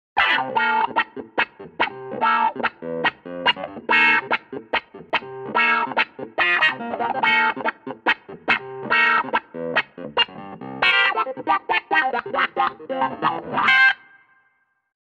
Record insane funky keyboard tracks - Best Session Musicians service
Link to this gig:
Record insane funky keyboard tracks Special discount
Best cheap Session Musicians Service
Hey guys, I'm a professional session musicians based in Rome, Italy. I have been touring with bands for the last 10 years and developing my own sound with every show.
I can't wait to record some FUNKY groovin' tracks for you.

Keyboards,Soul,Funk